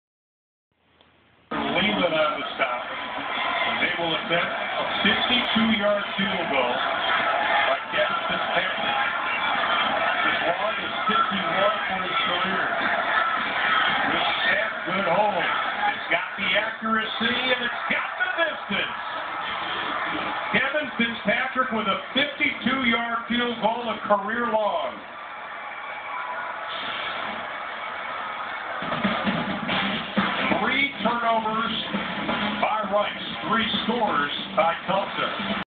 Kevin Fitz drills a 52 yard field goal
Kevin crushes a 52 yarder. The kid makes it look easy.